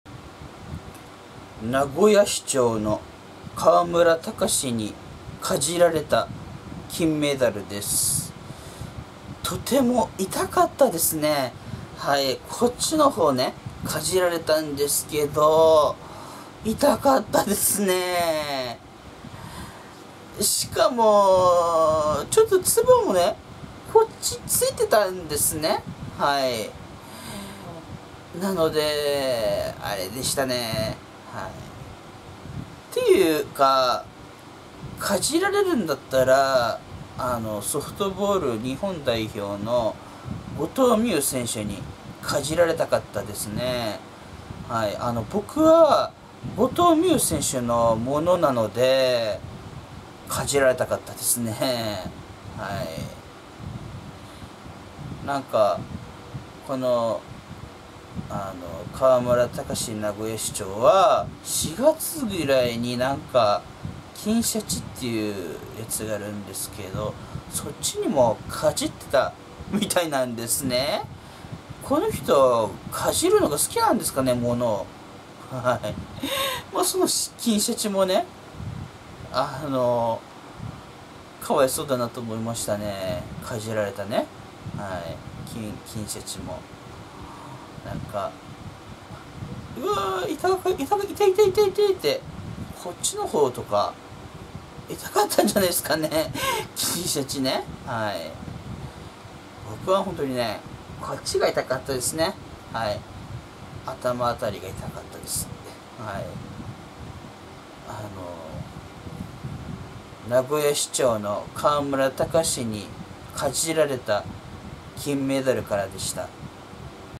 名古屋市長の河村たかしにかじられた金メダルです。とても痛かったです。（金しゃち 金のしゃちほこ 後藤希友）
名古屋市長の河村たかしさんがソフトボール日本代表の後藤希友選手の金メダルをかじったことについてですが、4月には金しゃちをかじっていたようです。